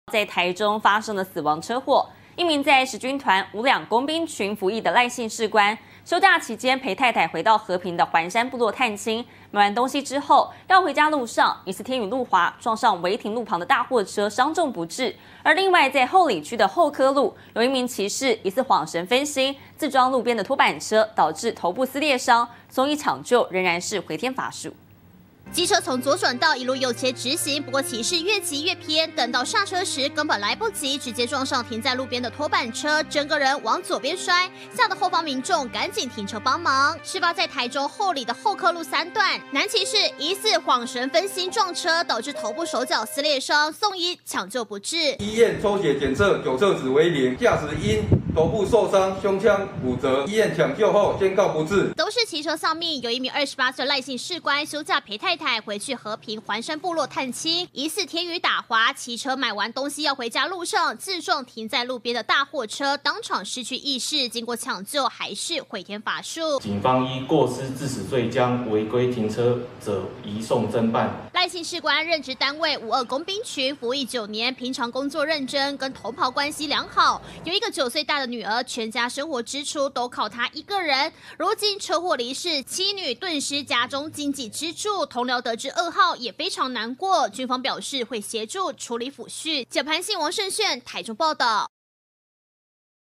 騎士疑恍神分心撞拖板車 頭部撕裂傷不治
接下來帶您來看到，台中兩起死亡車禍，一名在十軍團五二工兵群服役的賴姓士官，休假期間陪太太回到和平的環山部落探親，在買完東西要回家的路上，疑似下雨打滑，自撞違停在路邊的大貨車，傷重不治，另外在后里區后科路，還有一名騎士因疑似恍神分心，自撞路邊的拖板車，導致頭部撕裂傷，送醫搶救仍回天乏術。